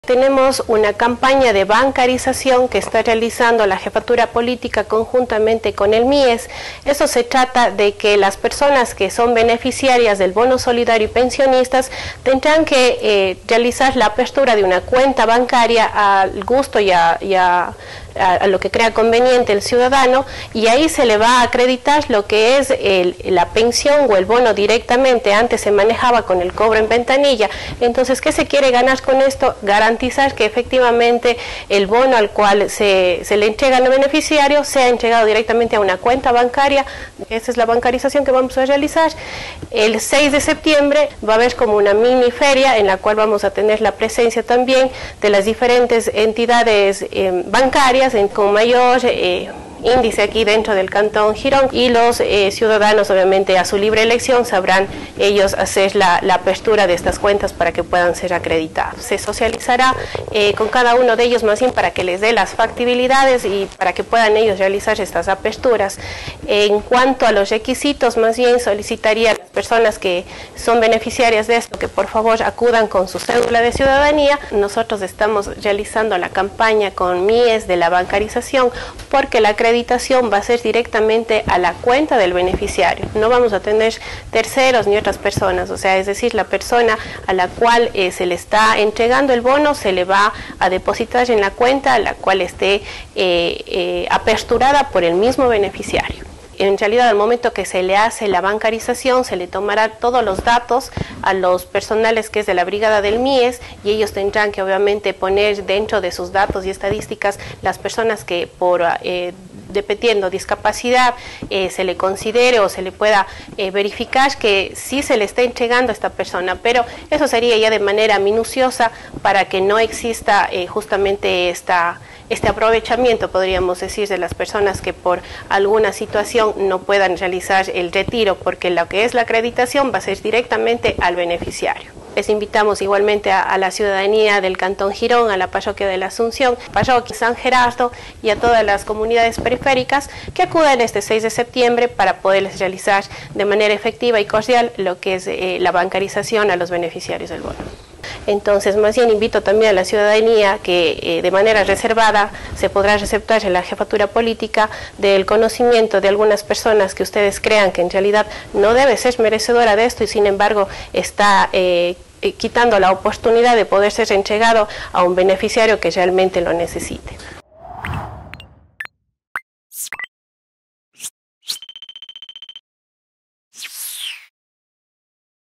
Beneficiarios de bonos del Estado deben tener una cuenta bancaria
A partir de octubre de 2024, las personas beneficiarias de los bonos deben contar u obtener una cuenta bancaria y registrarla en el MIES para recibir las transferencias mensuales. Los beneficiarios recibirán los bonos en cuentas bancarias y ya no en las "concentradoras" de las ocho instituciones bancarias con las que el MIES tiene un convenio desde 2013. Para el efecto este 6 de septiembre en Girón se desarrollará la FERIA DE BANCARIZACIÓN dirigida a los usuarios del bono indicó la jefe política Jenny Ortiz.